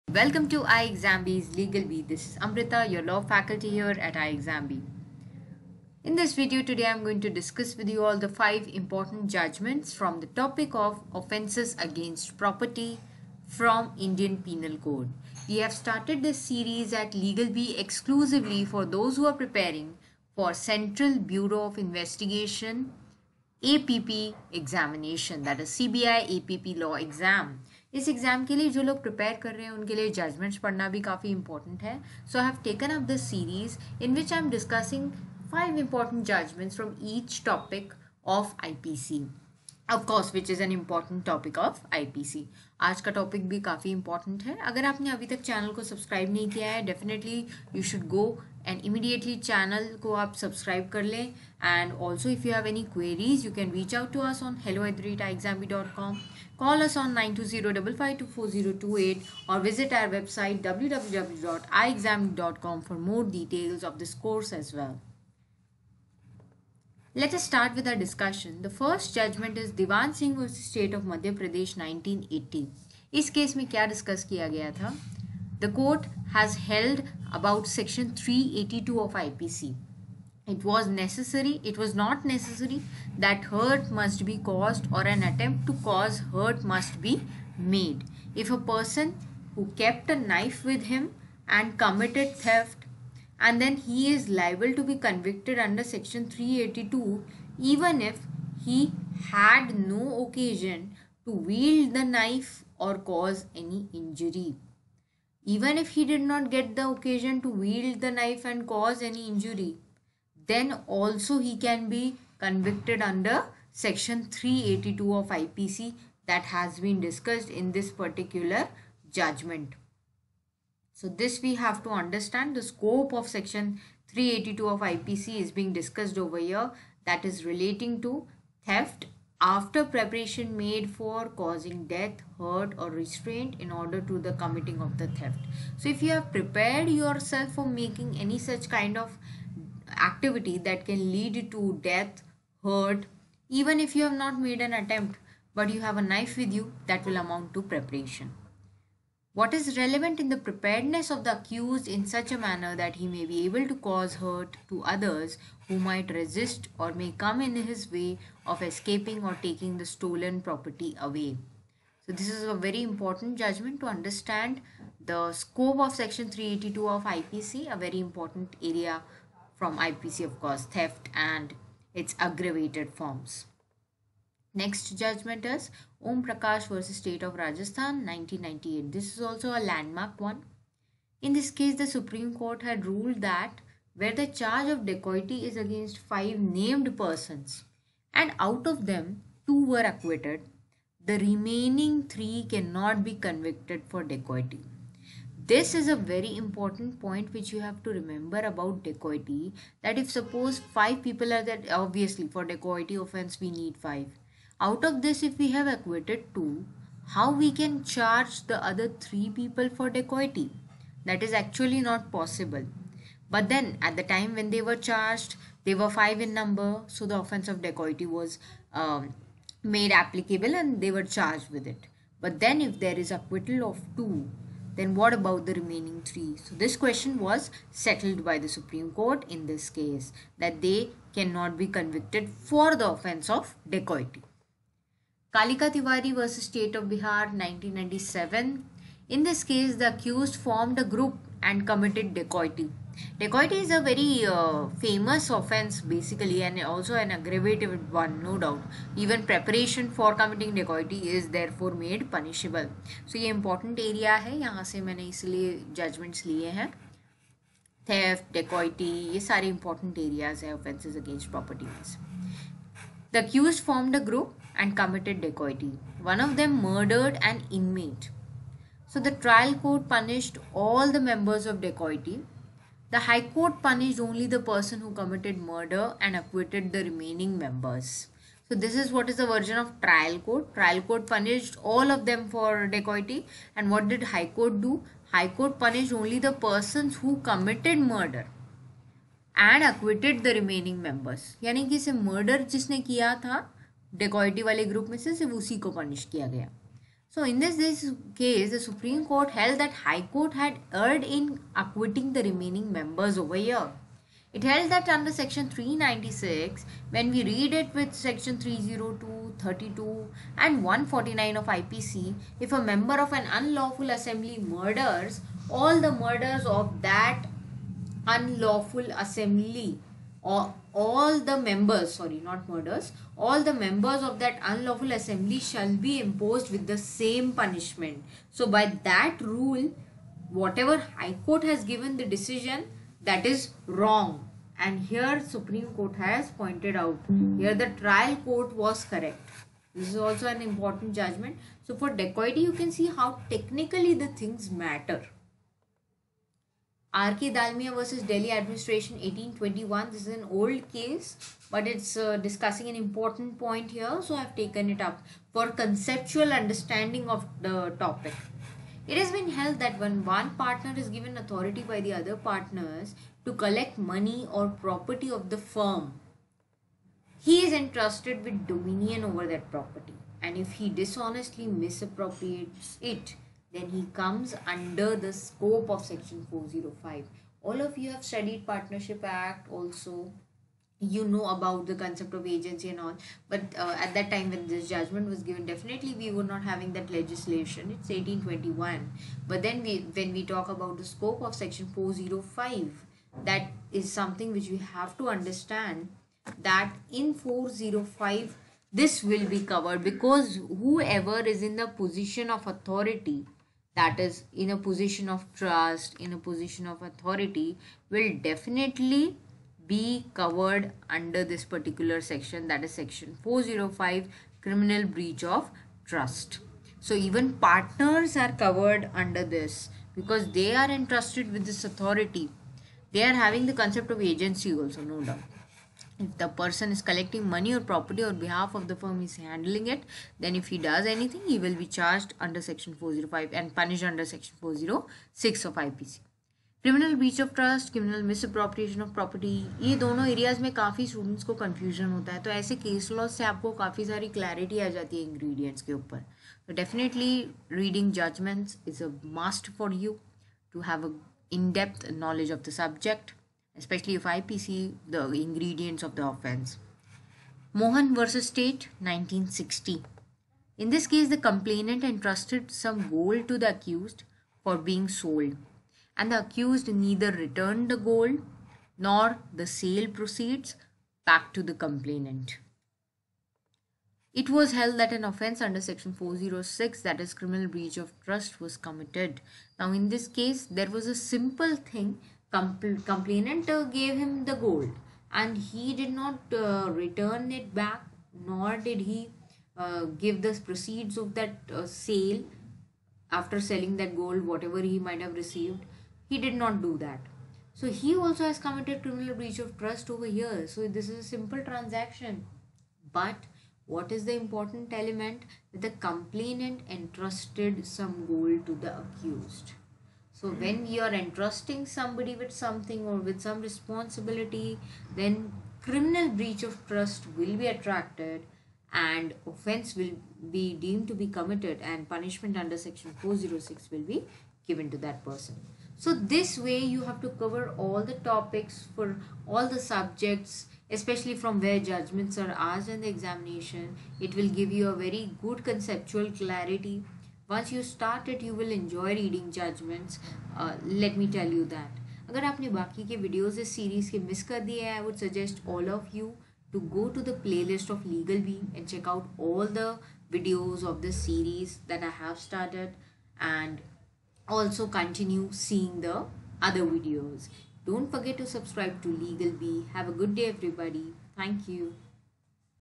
CBI 2023 | CBI APP 2023 | 5 Important Judgments from IPC (Offences against Property) | By Amrita Mam
Hello Everyone ,In this video Amrita maam has discussed 5 important judgments from the Indian Penal Code topic of Offences against Public Tranquility. Those who are preparing for the CBI APP Law exam must be comprehensive in their preparation and must be prepared with important judgments as well. The judgments are being dealt with conceptually and topic wise in this series. Watch all the videos of this series and take advantage of the same.